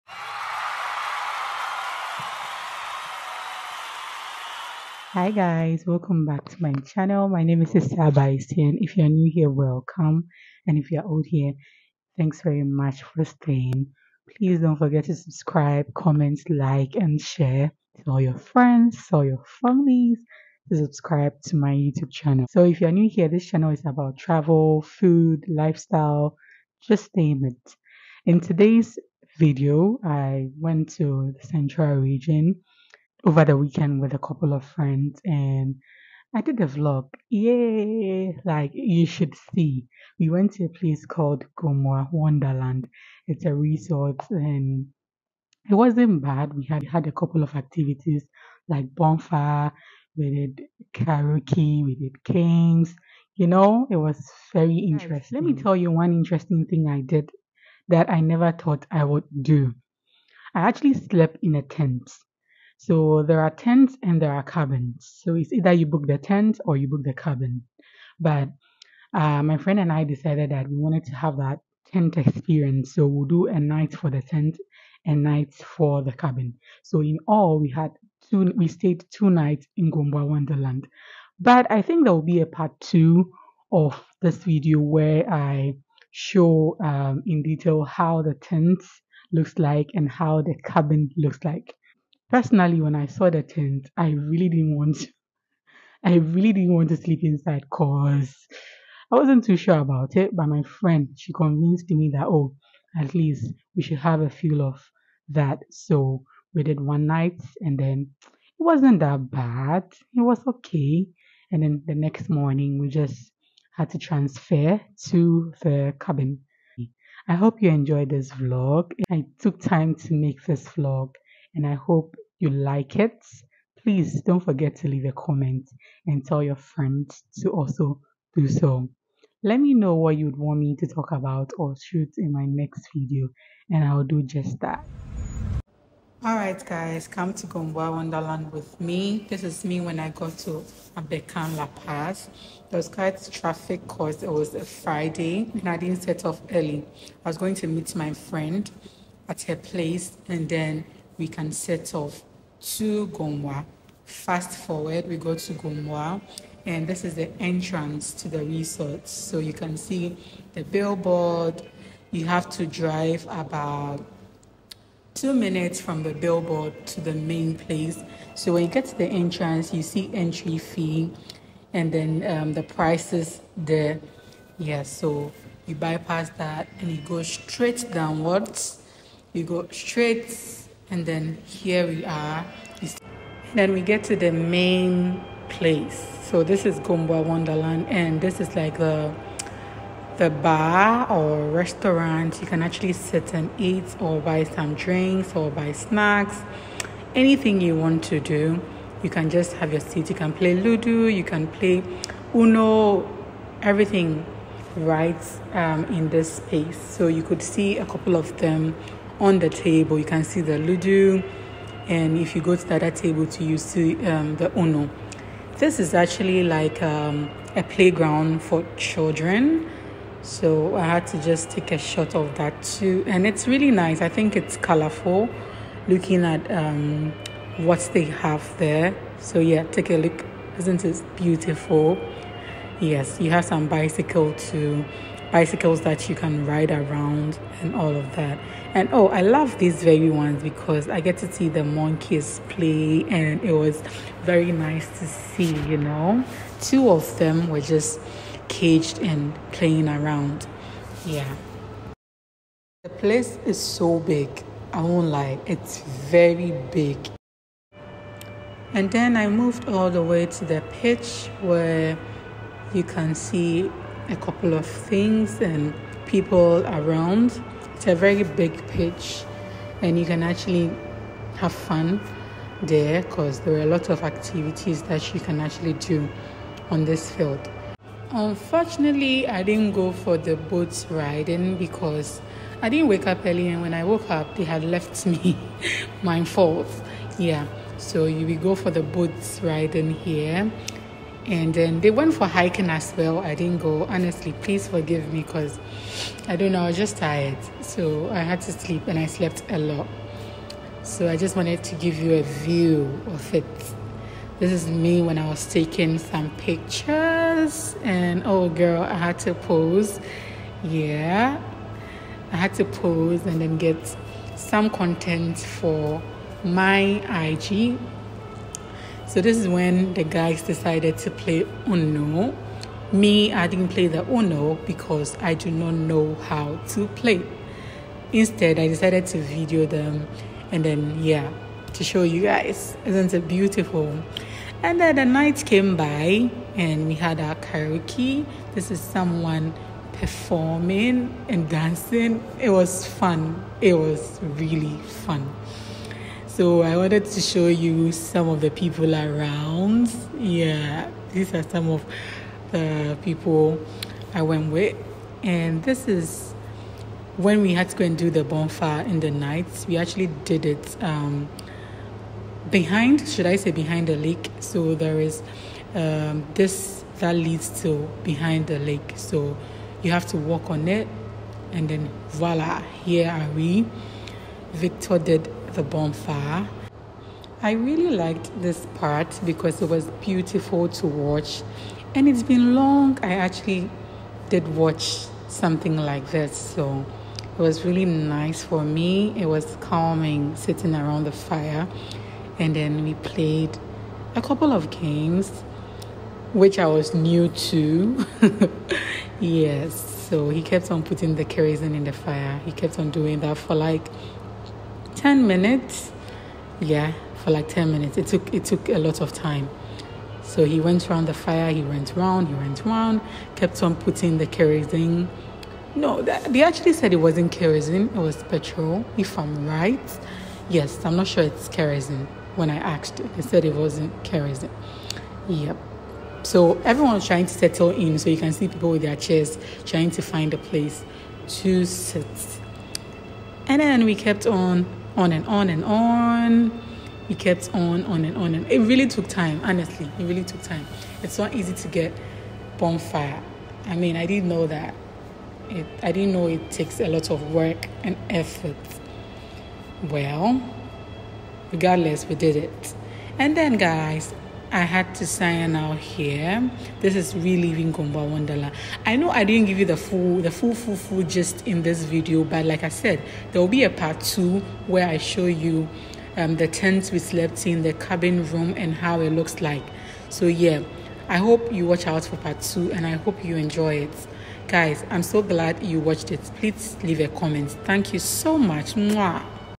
The Gomoa Wondaland Experience/Travel Vlog . pt1
Hello loves ,

My weekend was amazing , had to “getaway” 😅😅 and it was worth it. In this video I show you around Gomoa wondaland and what to expect when you visit there . Enjoy

More amazing content coming up .

Xoxo
E.